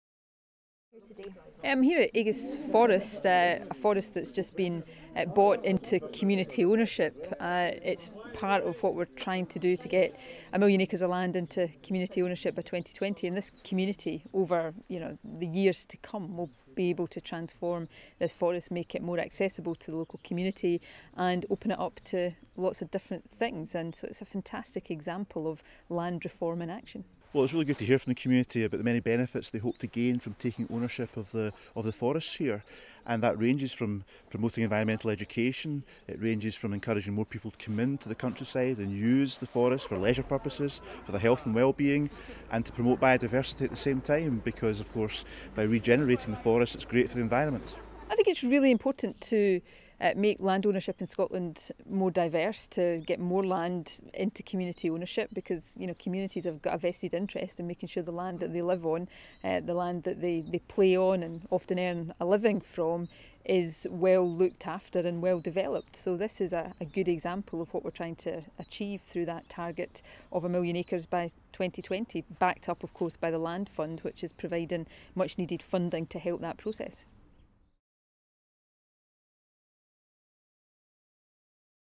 Success of land reform
Over 45,000 people in 32 communities are benefiting from land bought with help from the Scottish Land Fund, First Minister Nicola Sturgeon said today as two additional areas received funding.

Residents in Newburgh, Fife have been awarded £51,925 to buy a former town reservoir and associated land while the residents of Culbokie, on the Black Isle, will receive £96,691 to buy local land for community uses, following an announcement from the Scottish Land Fund today.